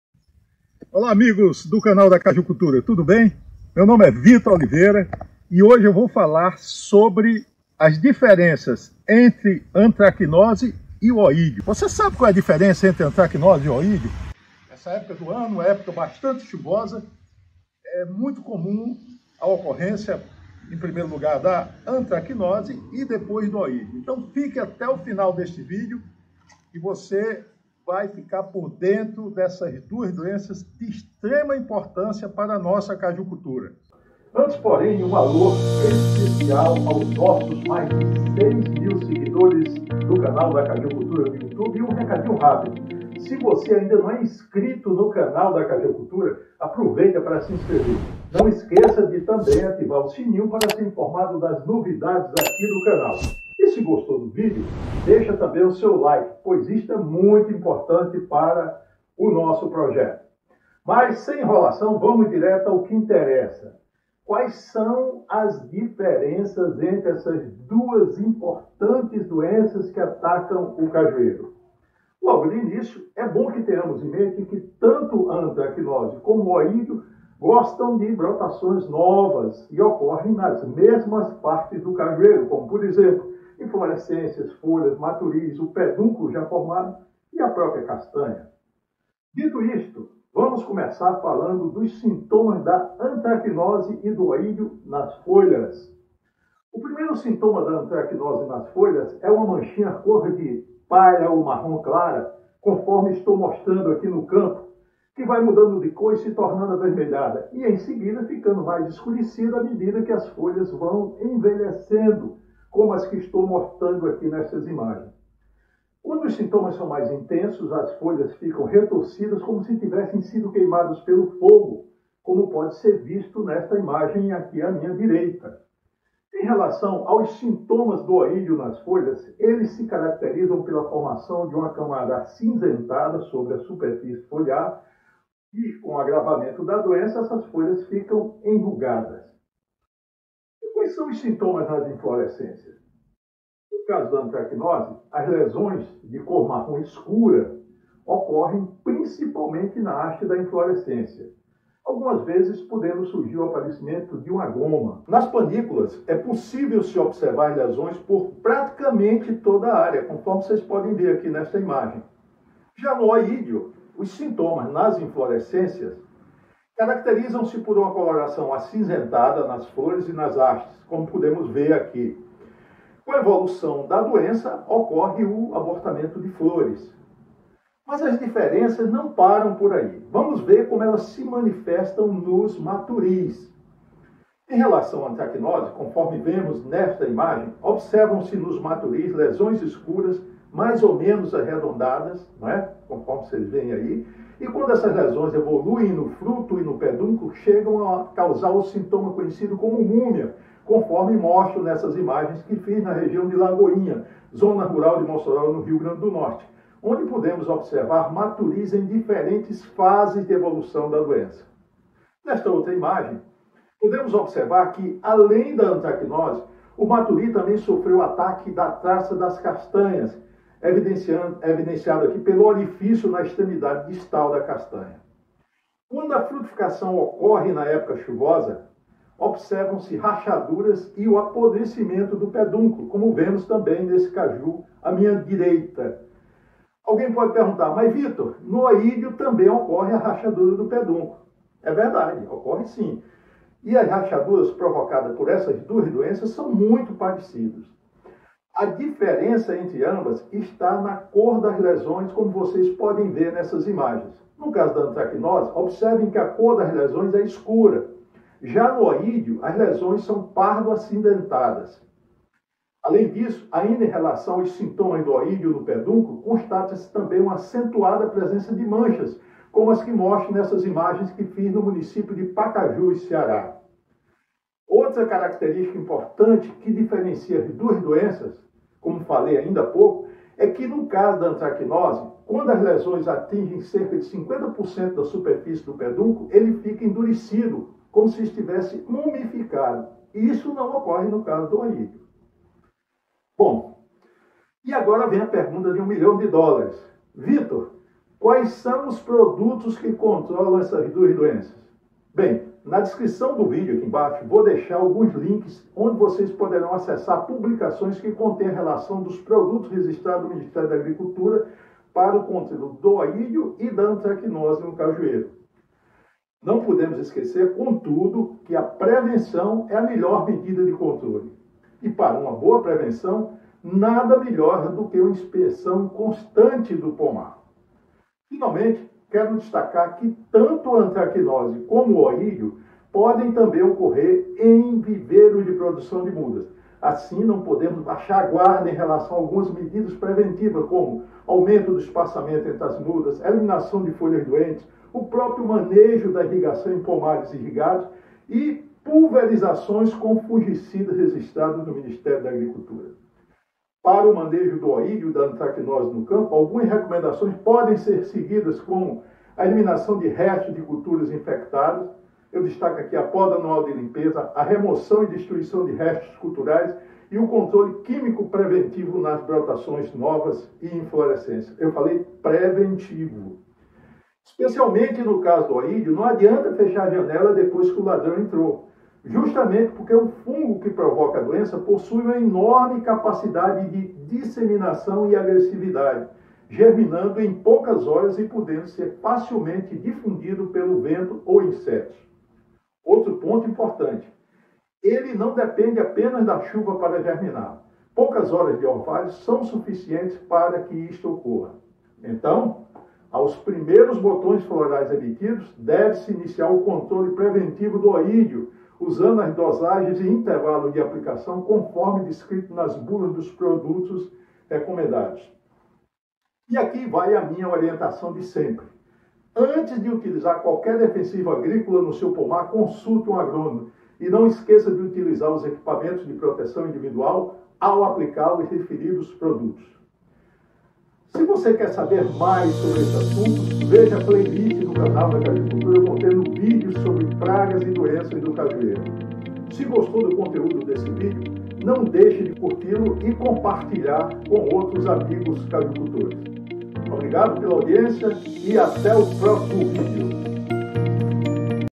Antracnose e Oídio do Cajueiro: aprenda a identificá-los.
Você sabe diferenciar os sintomas da antracnose e do oídio no cajueiro? Neste vídeo, o consultor em cajucultura Vitor Oliveira apresenta os principais sintomas que diferenciam essas duas importantes doenças que ocorrem no cajueiro.
Saiba mais:
- Nos links a seguir encontram-se tabelas com a relação dos defensivos agrícolas registrados para o cajueiro, dentre os quais encontram-se produtos indicados para o controle da antracnose e do oídio
Recentemente foi registrado mais um produto para o controle da antracnose no cajueiro, o Bravonil Top, o qual não consta nas relações acima.